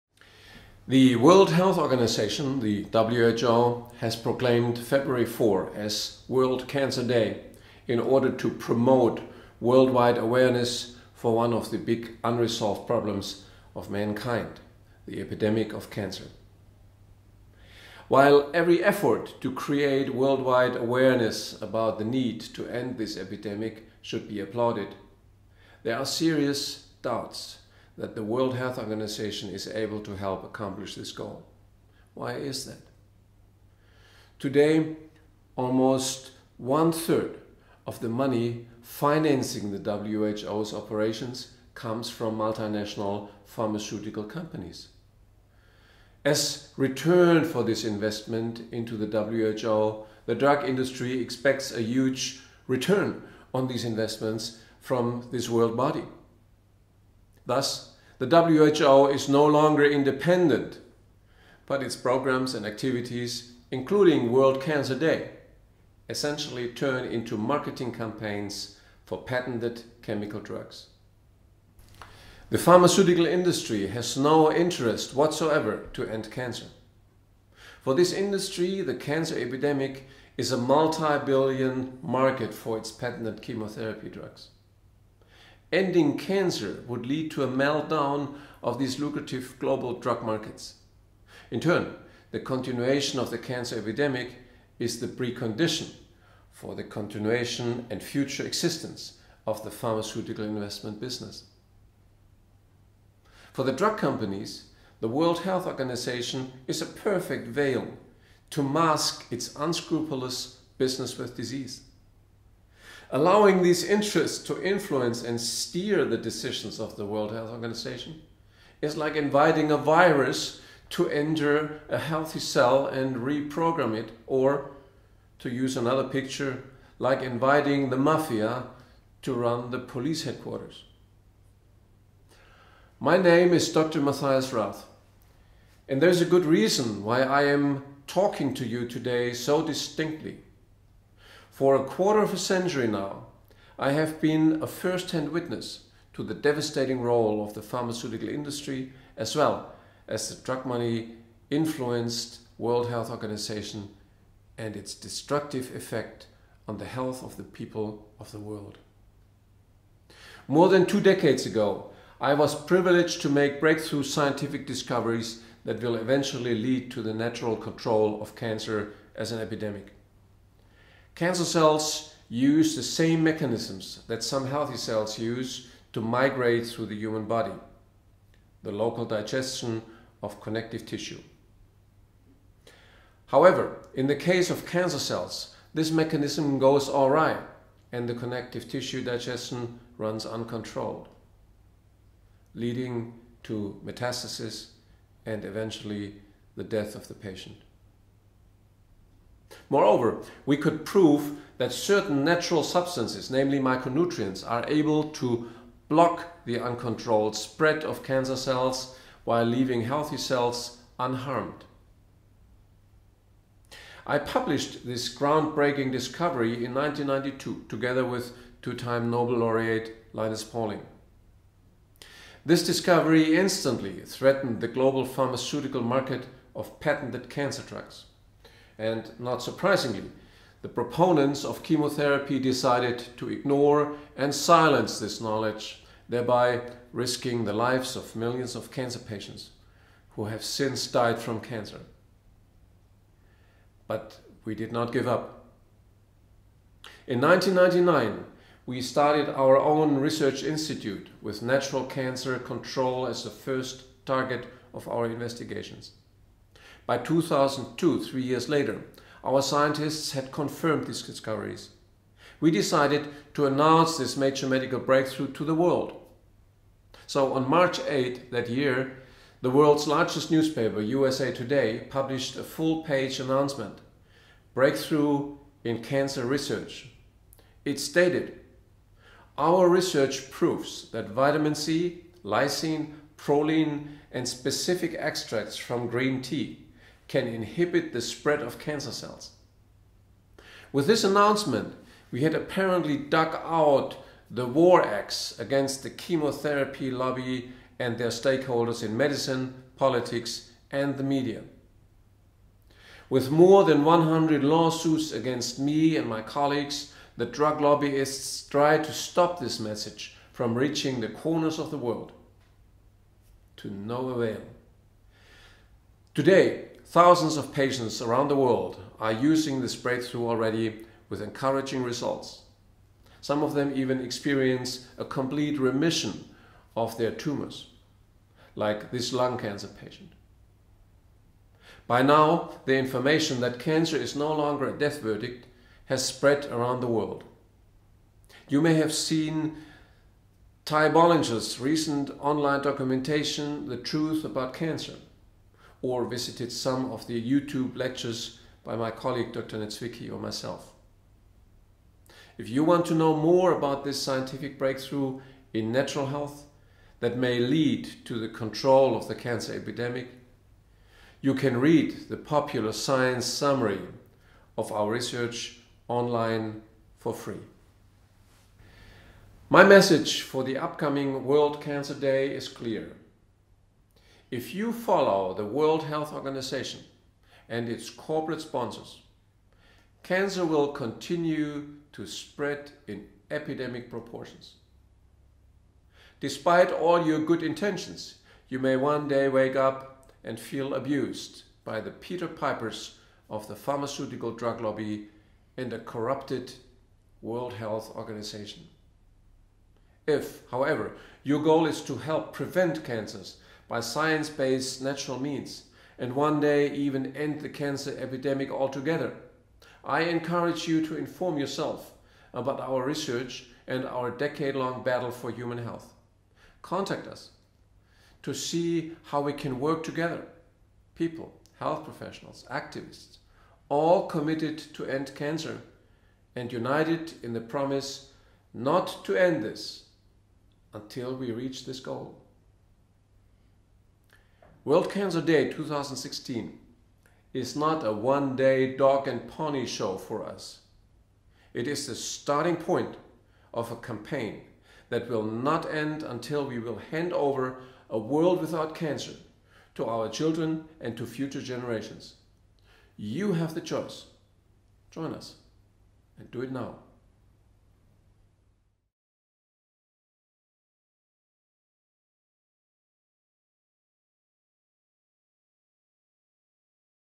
World Cancer Day: You Have The Choice, Join Us And Do It NOW! - Dr Matthias Rath
The World Health Organization – the WHO – has proclaimed February 4 as World Cancer Day in order to promote worldwide awareness for one of the big unresolved problems of mankind – the epidemic of cancer.

While every effort to create worldwide awareness about the need to end this epidemic should be applauded, there are serious doubts that the WHO is able to help accomplish this goal. Why? Today, almost one third of the money financing the WHO’s operations comes from multinational pharmaceutical companies. As ‘return’ for this ‘investment’ into the WHO, the drug industry expects a huge ‘return on its investments’ from this world body. Thus, the WHO is no longer independent but its programs and activities – including ‘World Cancer Day’ – essentially turn into marketing campaigns for patented chemical drugs.

If you follow the WHO and its corporate sponsors, cancer will continue to spread in epidemic proportions. Despite all your good intentions, you may one day wake up and feel abused by the ‘Peter Pipers’ of the pharmaceutical drug lobby and a corrupted WHO.

But if your goal is to help prevent cancers by science-based natural means – and one-day even end the cancer epidemic – we encourage you to inform yourself about our research and our decade-long battle for human health. Contact us to see how we can work together – people, health professionals, activists – all committed to ending cancer and united in the promise not to stop until we reach this goal.